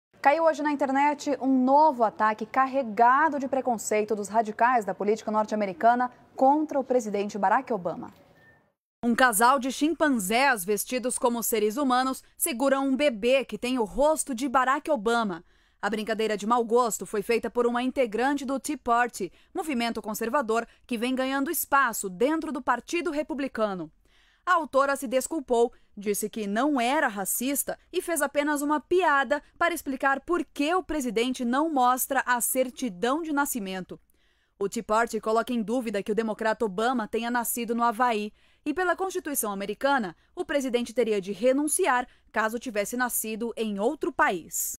Cai na internet ataque contra o presidente Barack Obama
O ataque, preconceituoso, mostra o presidente Barack Obama no colo de um casal de chimpanzés. A autora se desculpou e fez apenas uma piada para explicar o motivo pelo qual o presidente não mostra a certidão de nascimento.